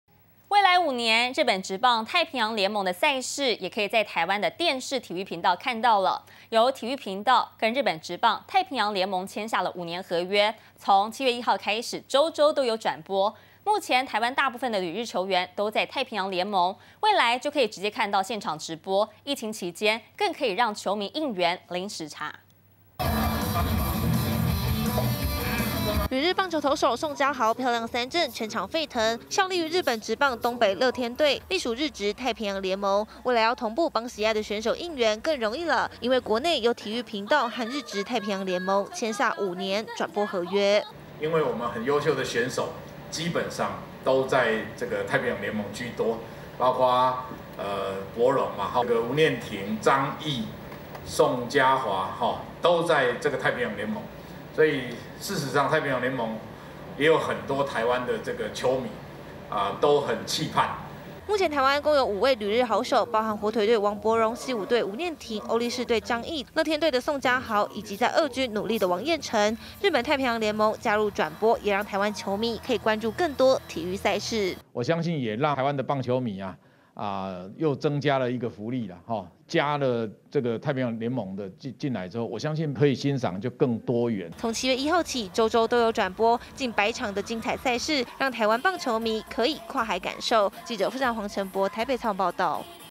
跨海應援！ 日職太平洋聯盟轉播"台灣能收看"
未來五年，日本職棒太平洋聯盟的賽事，也可以在台灣的電視體育頻道看到了。有體育頻道和日本職棒太平洋聯盟簽下5年合約，從7月1日起，週週都有轉播，目前台灣大部分的旅日球員都在太平洋聯盟，未來就可以看到現場直播，疫情期間，更可以讓球迷應援零時差。